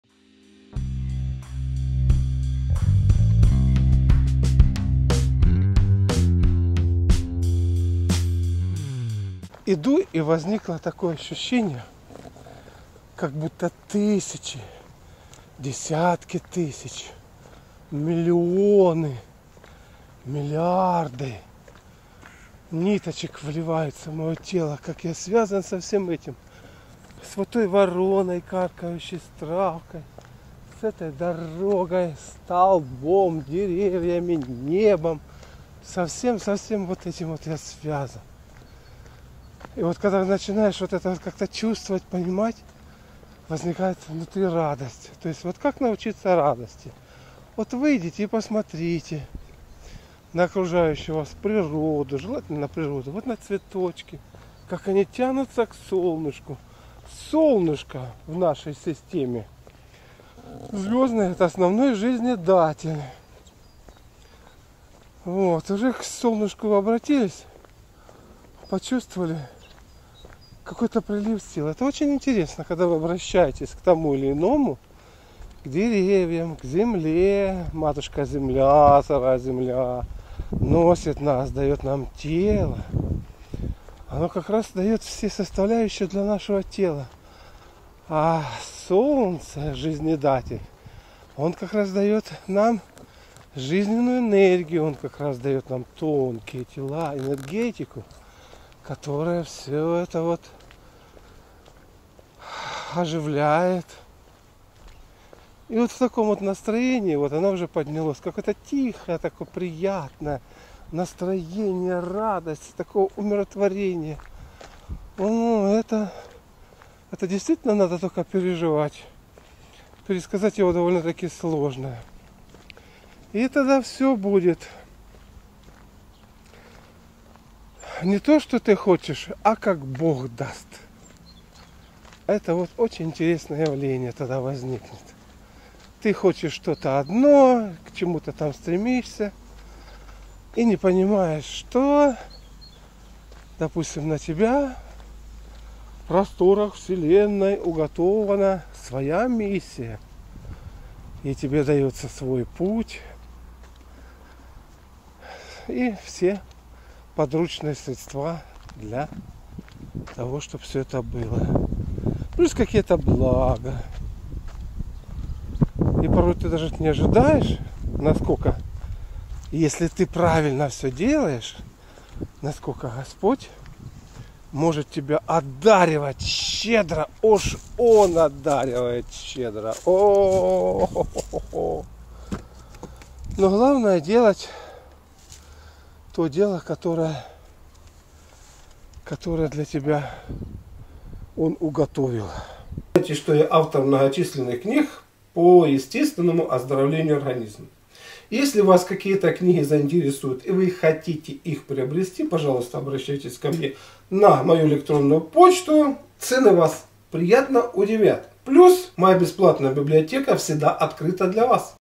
БОГ ДАСТ. Малахов.
Бог даст … очень емкое выражение. И у каждого человека есть свое понимание того, что Бог даст.